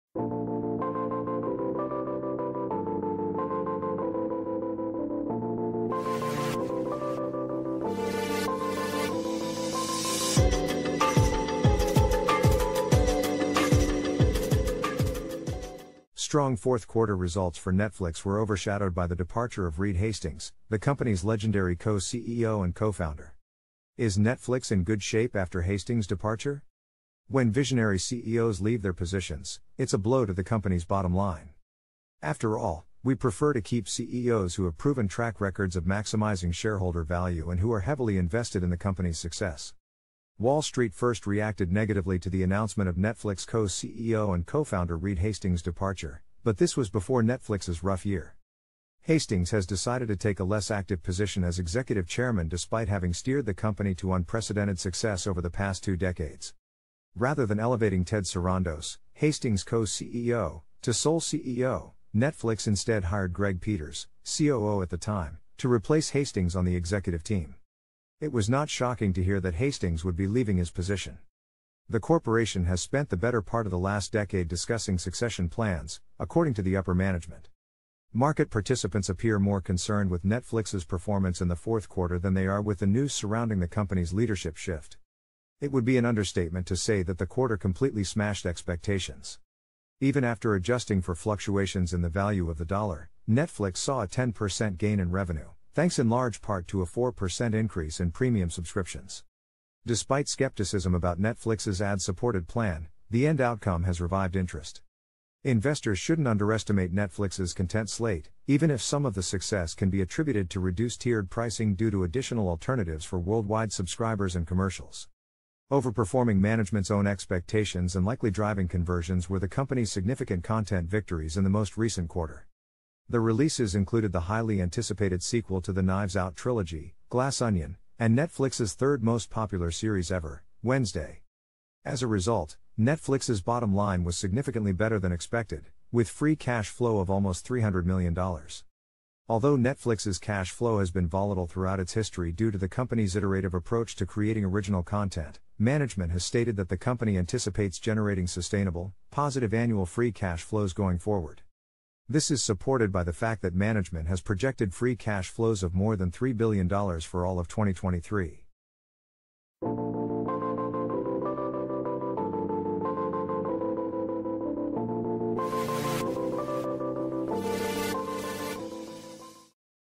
Netflix Is Evolving Into Something New
Netflix is not collapsing. They are simply shape-shifting. Name & All.